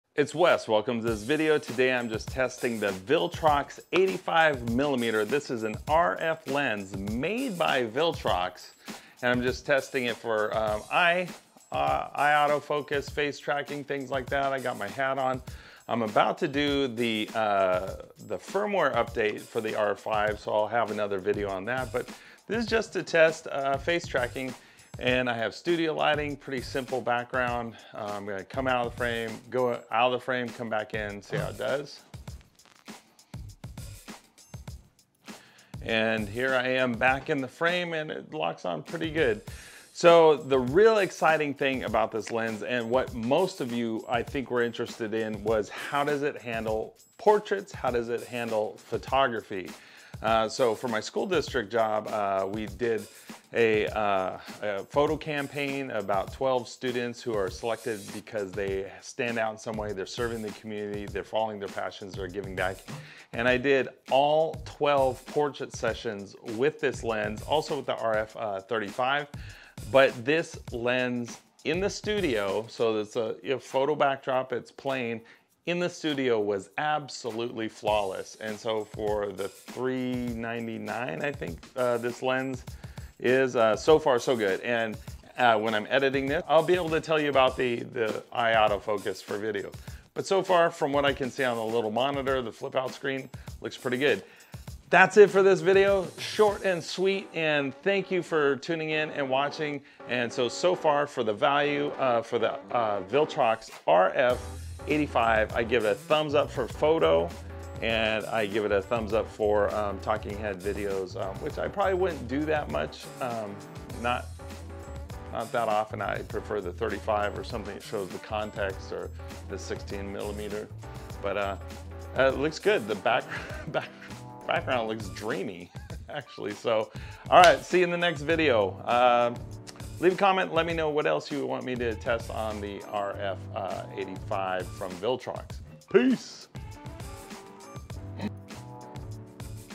First Impressions of the Viltrox RF85mm F/1.8 Lens for the Canon R5, R6, R or RP
The Canon R5
RF 16mm
RF 50mm F/1.8
RF 85 (by Viltrox)